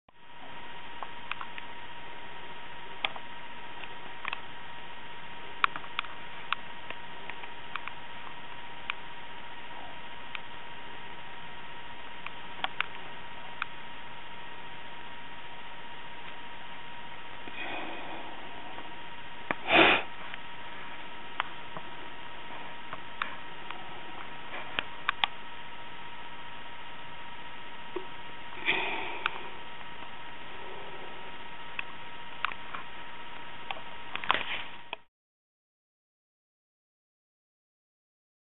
Swarmbees.m4v
Bees swarming (yes the little gritty things on camera)  in summer 2009